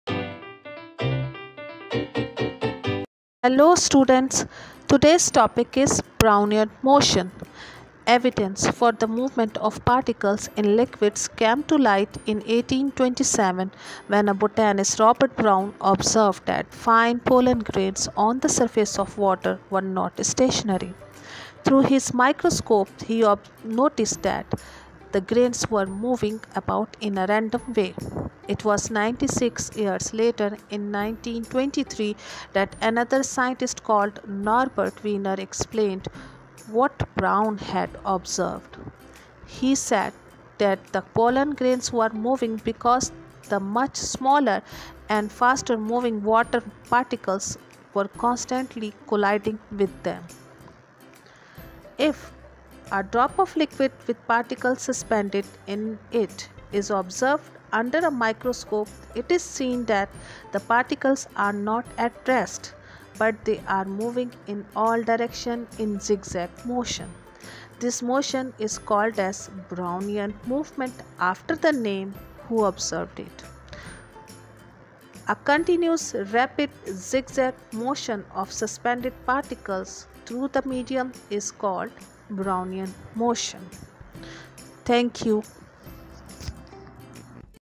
Brownian Motion | Robert Brown | Chemistry
Chemistry 🧪

Topic : " Brownian Motion "

Summary:

The random motion of visible particles caused by much smaller, invisible ones is called “Brownian motion” after the scientist who first observed this phenomenon.

00:00 Introduction

00:08  Robert Brown

00:15  Robert Brown's Pollen grains experiment

00:28  Norbert Wiener's Explanation on Robert Brown's Observation

01:14 Definition of Brownian Motion

Gas laws will be discussed in my next video 📺

Pollen grain experiment | Definition of Brownian motion | What is Brownian Motion | Water Particle | Chemistry

Lecture by :
Sehrish Naimat